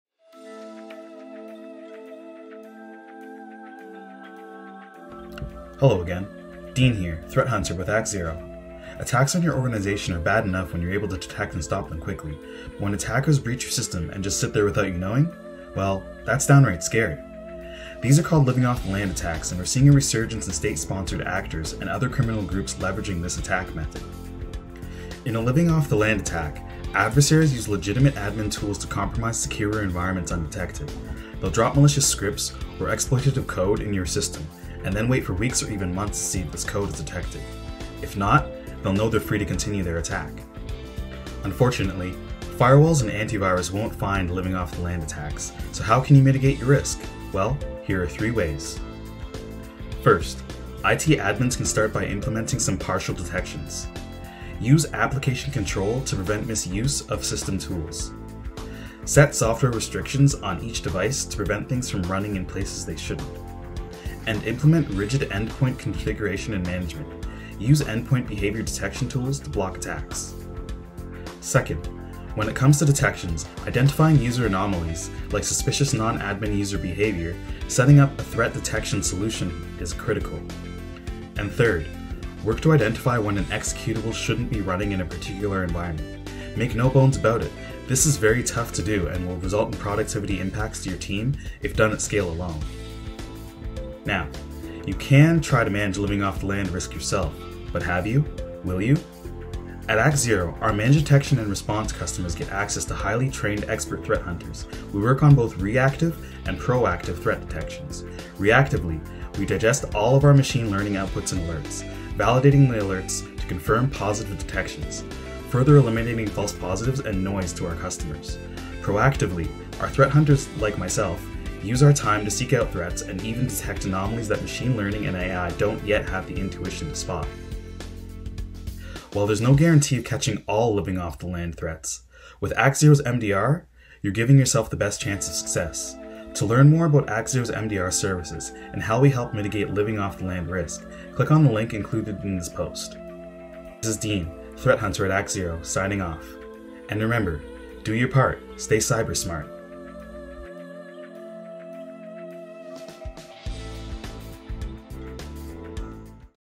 What You Need to Know About “Living off the Land” Attacks and How to Mitigate Them
Attacks on your organization are bad enough when you're able to detect and stop them quickly. But when attackers breach your system and just sit there without you knowing?; well, that's downright scary.

These are called "Living off the Land" attacks, and we're seeing a resurgence in state-sponsored actors and other criminal groups leveraging this attack method. How can you manage the risk?